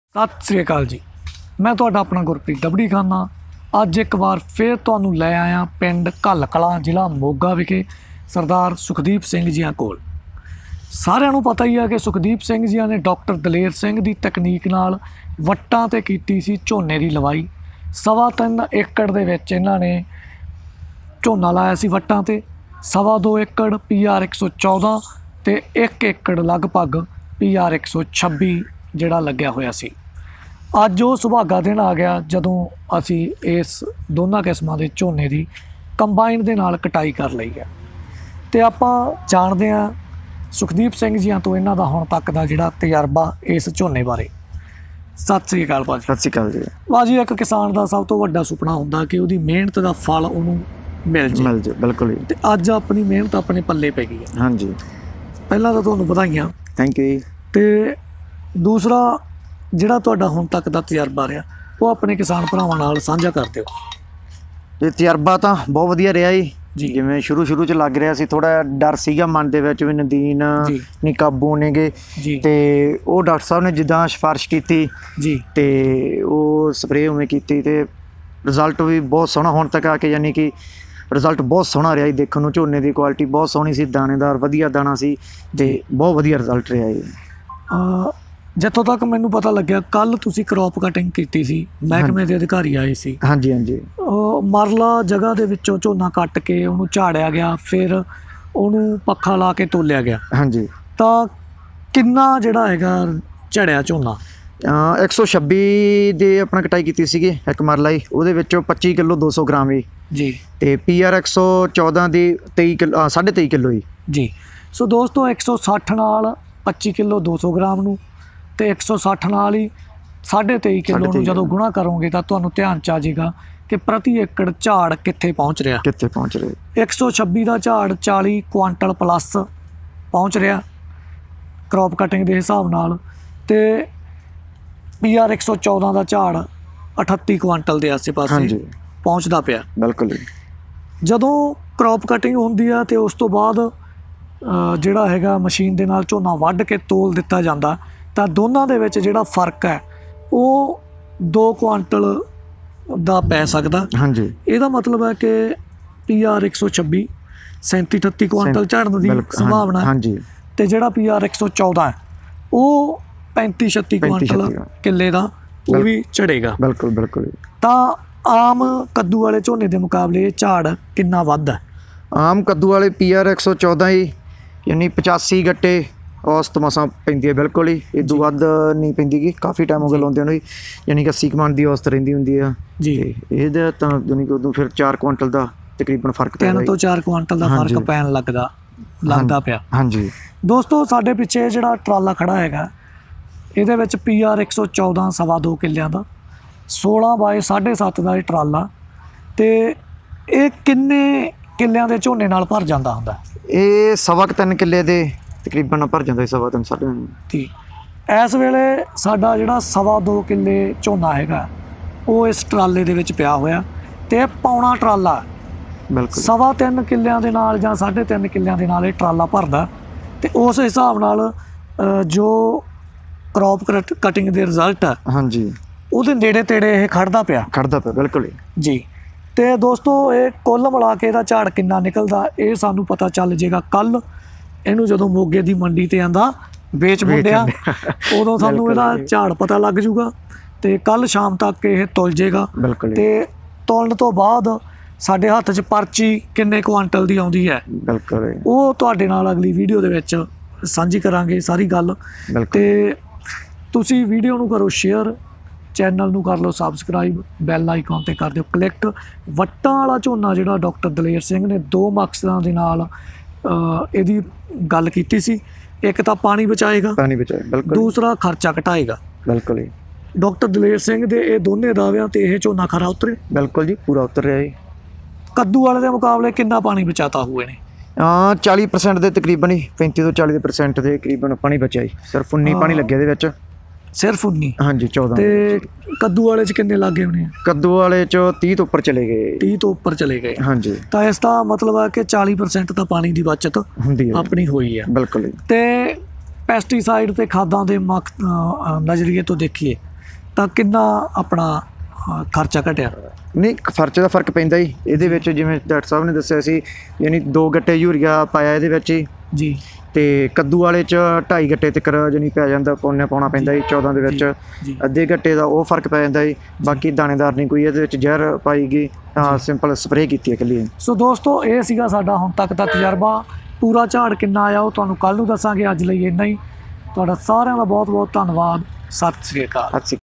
PaddyOnRidges ਵੱਢਿਆ ਗਿਆ ਵੱਟਾਂ ਵਾਲਾ ਝੋਨਾ/कट गया मेंढ़ों वाला धान
Paddy on Ridges cultivated at village Hall Kalan of district Moga, Punjab (India) has been harvested.
For gaining the detailed information about the crop please watch this video till the end.